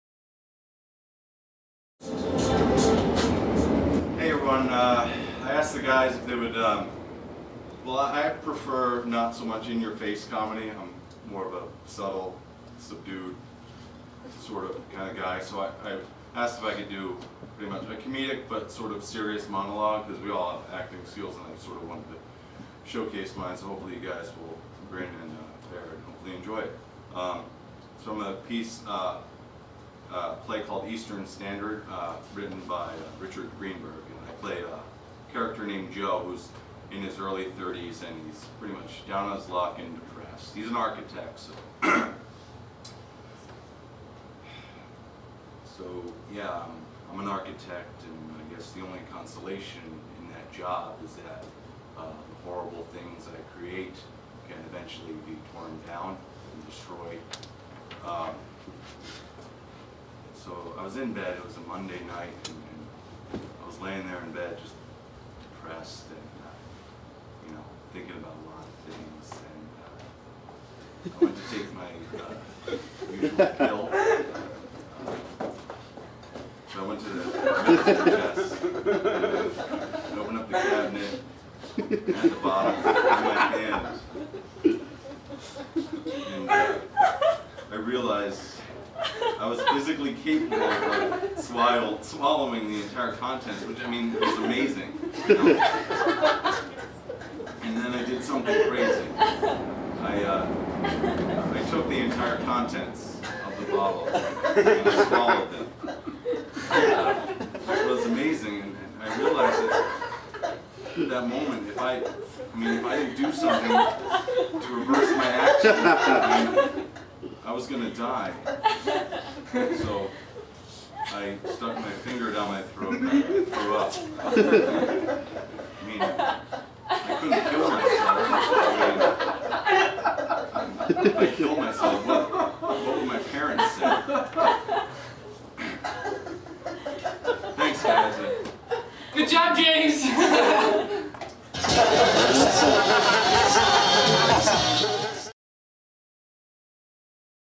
Serious Monologue - 04.08.09
Comedy Accident Presents... Serious Monologue! Filmed 04.08.09 at The Apollo Theater in Chicago Illinois.